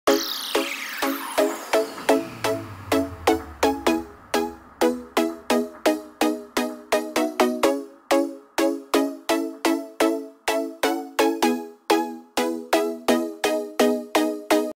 Amazing Alien Mystery Solved by Geoff Hunt
In this video, I (Geoff Hunt) show highlights of DJ Alien Mystery Solved in one of his performances.